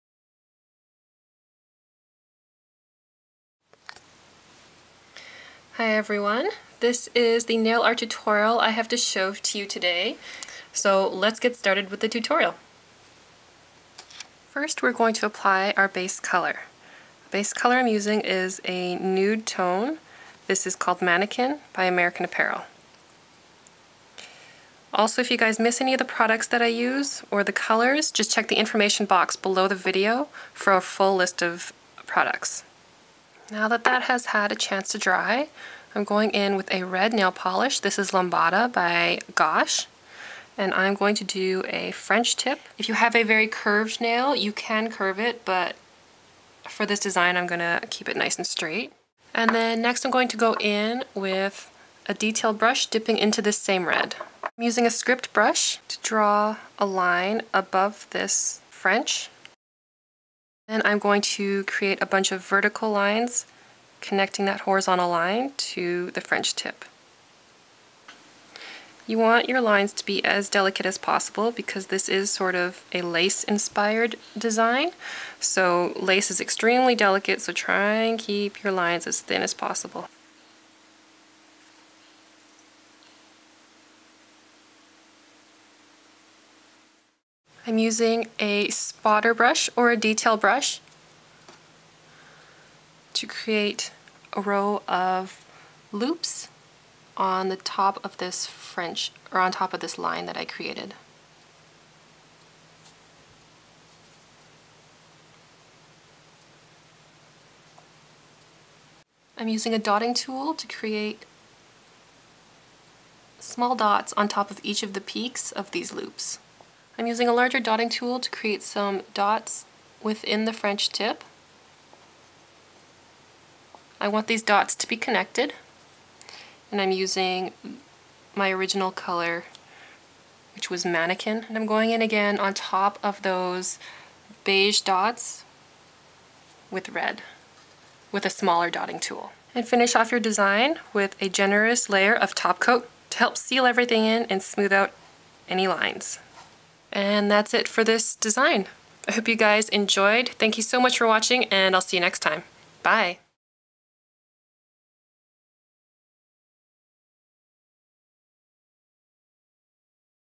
Red Lace French - Free Hand Nail Art Tutorial
Geeze it's been a while.
Well, I'm back with a new free hand tutorial!  - a lacey red french tip design.  Enjoy!
Materials Used:
OPI - Natural Nail Base Coat (Not Shown)
American Apparel - Mannequin
Gosh - Lambada
Dynasty - Script Brush size 10/0 (can be bought at your local art store in the acrylic paint brush section
American Painter - Spotter Brush size 5/0  (also found at the art store)
Dotting tool - multiple sizes.  can be bought on ebay
Sally Hansen - Insta Dry Anti Chip Top Coat